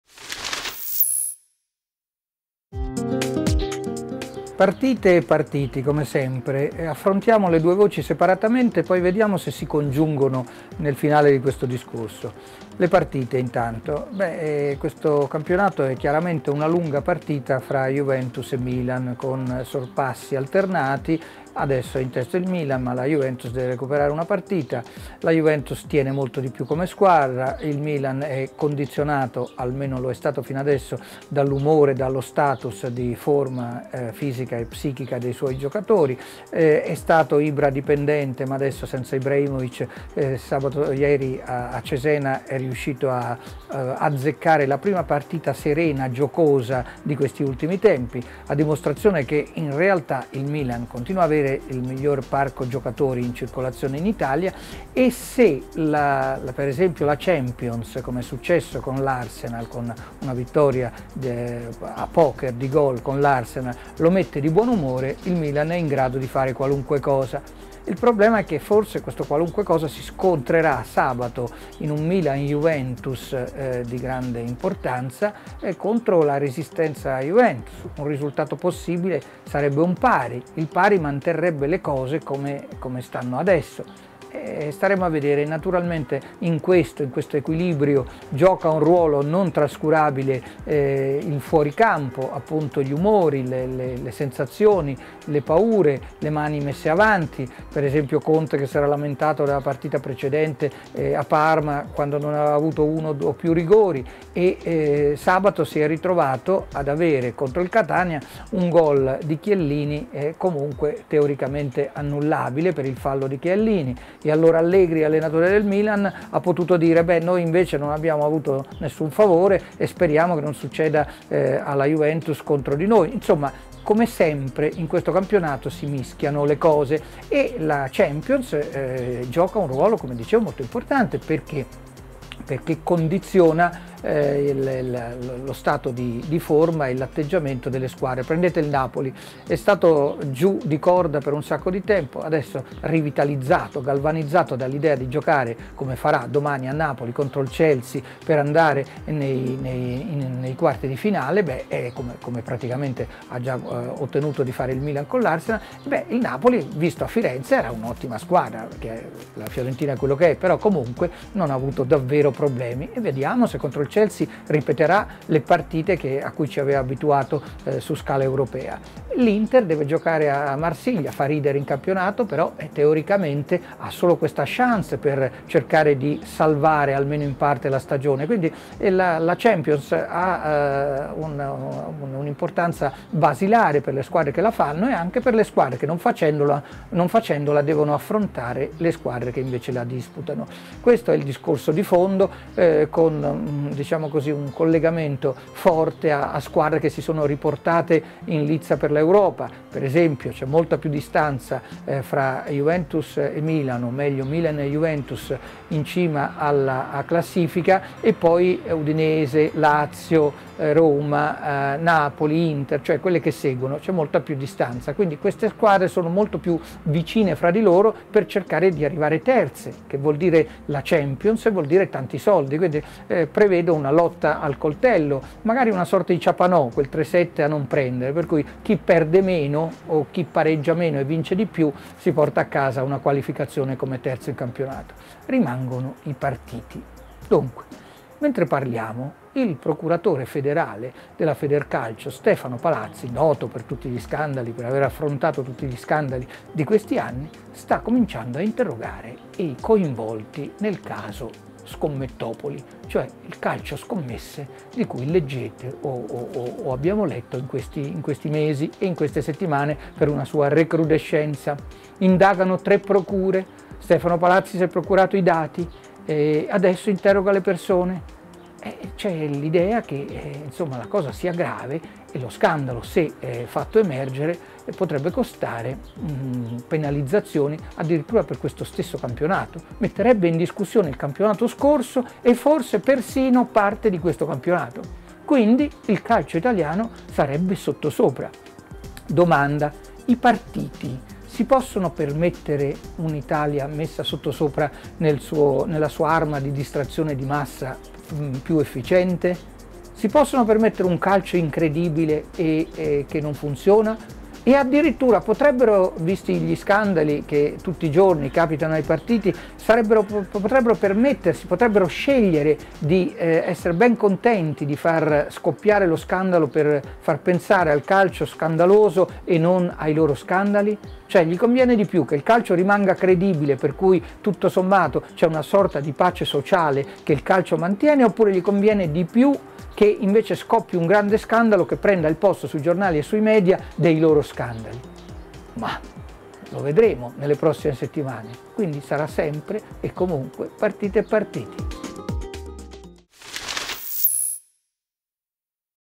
Beha: "Milan-Juve fino in fondo, in attesa della giustizia sportiva"
| Il campionato se lo giocano Milan e Juve. Rossoneri in testa, al momento, ma i bianconeri hanno una partita in meno. La squadra di Allegri ha tanto talento, quella di Conte è più organizzata. Sabato prossimo, finalmente, lo scontro tra le prime della classe. Di sicuro, la Champions league condizionerà il campionato. Basta guardare il Napoli, rivitalizzato dalle sfide europee. Contro la Fiorentina, i partenopei hanno avuto vita facile. Domani, però, al San Paolo arriva Chelsea. All'Inter, invece, resta solo la Champions, se vuole salvare la stagione. Il procuratore della Federcalcio Stefano Palazzo, intanto, sta iniziando ad interrogare i protagonisti della vicenda 'Scommettopoli'. Si rischiano penalizzazioni per il campionato passato e per quello in corso. Il sospetto, è che il bubbone stia per esplodere.
di Oliviero Beha